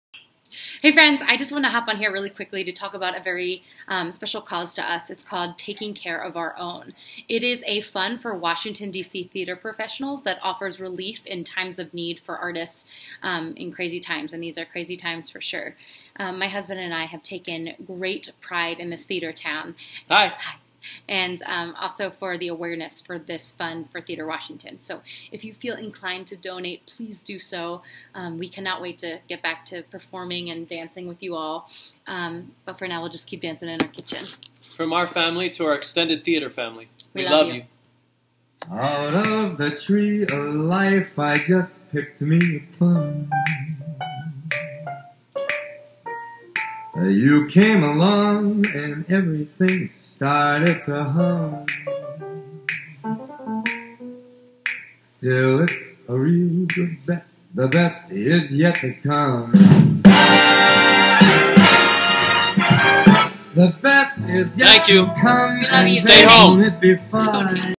Maria Rizzo & Kurt Boehm
Keeping dancing in your kitchen! Great advice and great moves from Maria Rizzo and Kurt Boehm. Thanks for being part of our VirtualBucketBrigade to raise emergency funding for professional theatre-makers in need at this time.

We're so grateful to the DC theatre artists who have participated in our VirtualBucketBrigade to help us raise funds for the Taking Care COVID-19 Emergency Relief Fund.